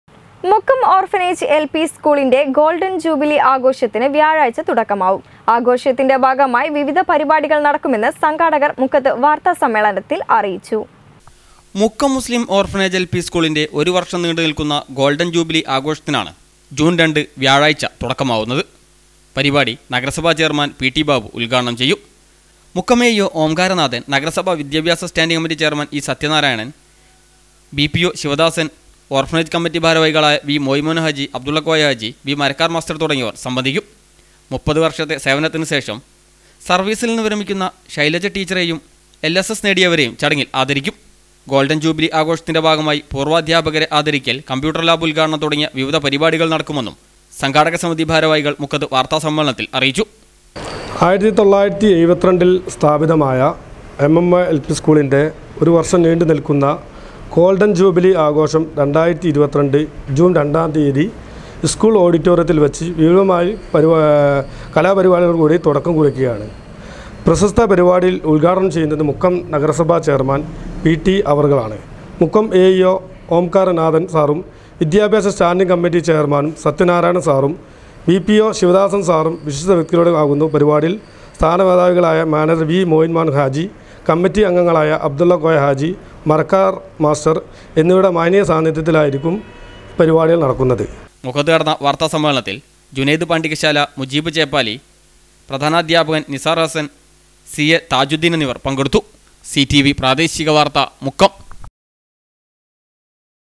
മുക്കം ഓർഫനേജ് എൽ.പി സ്കൂളിൻ്റെ ഗോൾഡൻ ജൂബിലി ആഘോഷത്തിന് വ്യാഴാഴ്ച തുടക്കമാവും
മുക്കം ഓർഫനേജ് എൽ.പി സ്കൂളിൻ്റെ ഗോൾഡൻ ജൂബിലി ആഘോഷത്തിന് വ്യാഴാഴ്ച തുടക്കമാവും. ആഘോഷത്തിൻ്റെ ഭാഗമായി വിവിധ പരിപാടികൾ നടക്കുമെന്ന് സംഘാടകർ മുക്കത്ത് വാർത്ത സമ്മേളനത്തിൽ അറിയിച്ചു